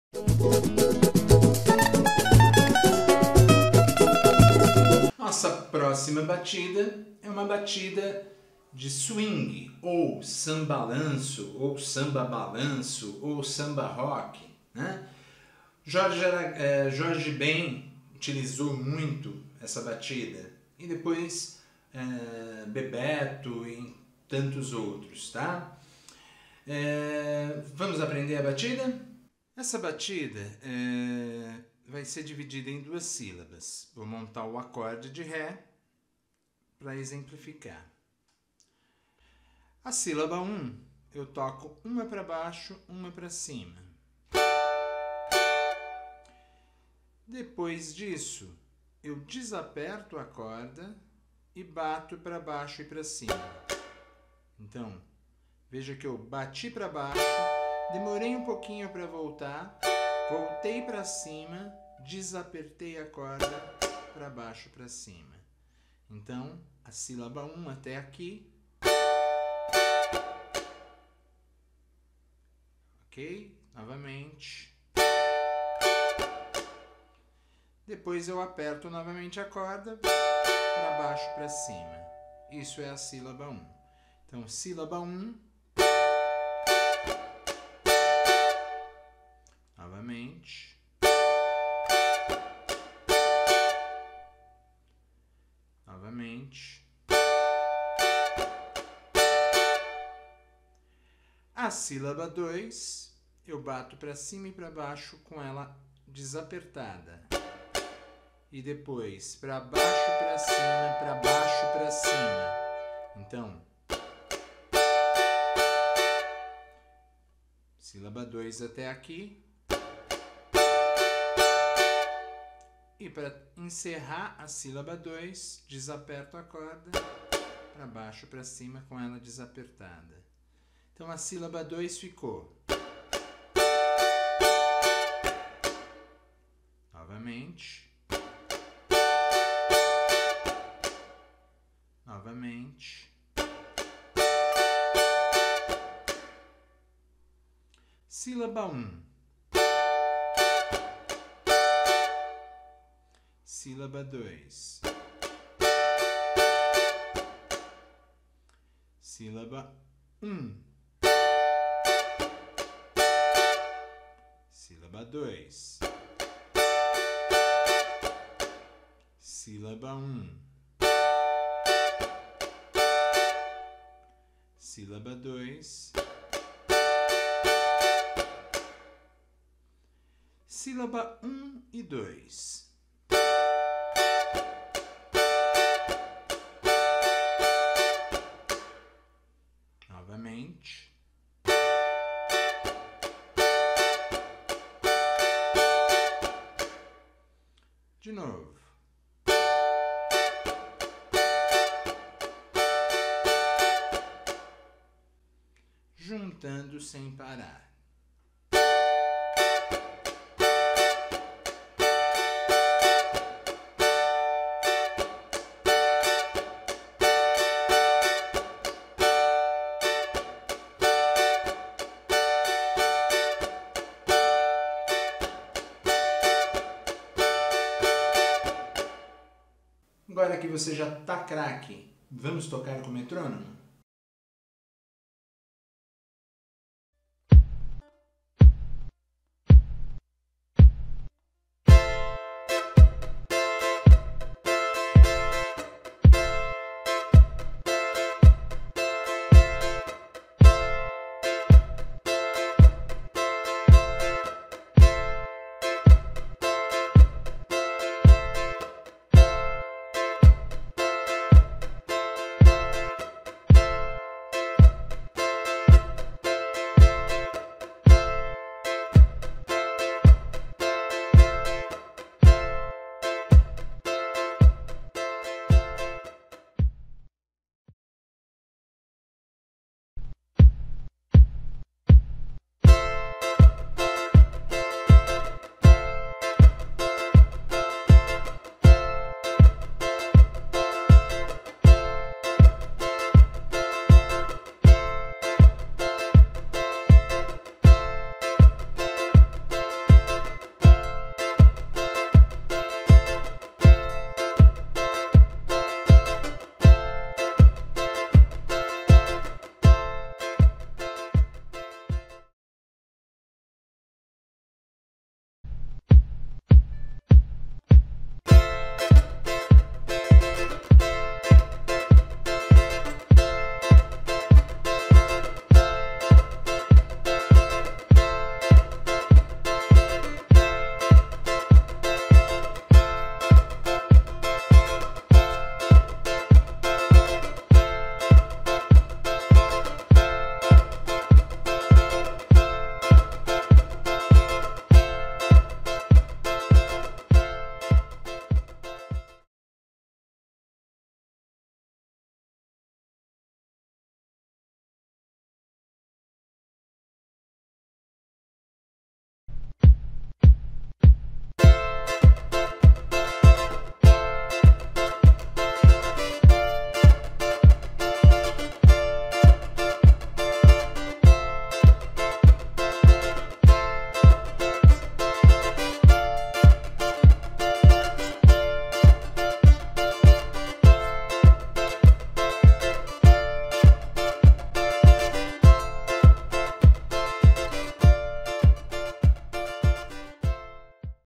Aula de batida de samba rock no cavaquinho
cavaquinhoprimeirosacordes Nesta aula de batidas de cavaquinho vamos aprender a tocar a batida de samba rock ou swing. Leia o artigo completo

Créditos:
Marcos Duprá - professor